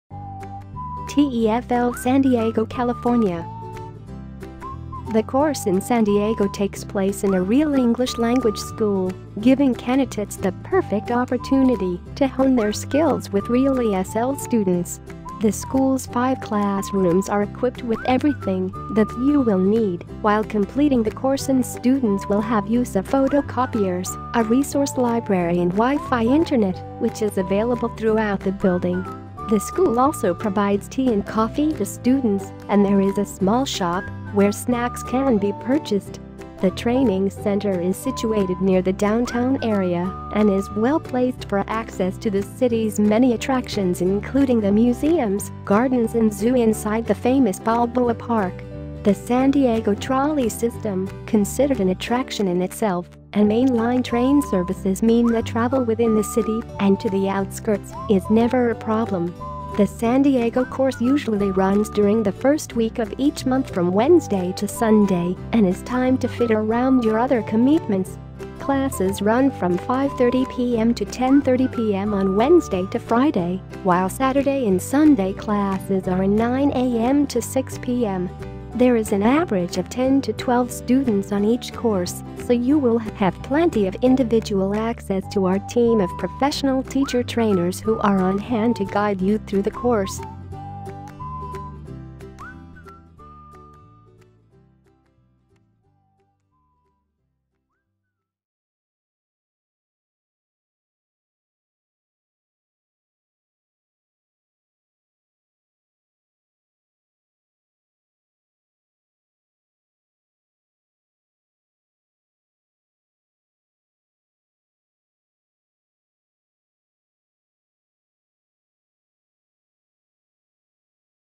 Welcome to Our TEFL / TESOL School in San Diego School | Teach & Live abroad!
The course in San Diego takes place in a real English language school, giving candidates the perfect opportunity to hone their skills with real ESL students. The school’s five classrooms are equipped with everything that you will need while completing the course and students will have use of photocopiers, a resource library and Wi-Fi internet, which is available throughout the building. The school also provides tea and coffee to students and there is a small shop where snacks can be purchased.

The training center is situated near the downtown area and is well placed for access to the city’s many attractions including the museums, gardens and zoo inside the famous Balboa Park. The San Diego Trolley system, considered an attraction in itself, and mainline train services mean that travel within the city and to the outskirts is never a problem.

The San Diego course usually runs during the first week of each month from Wednesday to Sunday and is timed to fit around your other commitments. Classes run from 5.30 p.m. to 10.30 p.m. on Wednesday to Friday while Saturday and Sunday classes are 9:00 a.m. to 6:00 p.m. There is an average of 10 to 12 students on each course so you will have plenty of individual access to our team of professional teacher trainers who are on hand to guide you through the course.

Find out more about taking a TEFL course in San Diego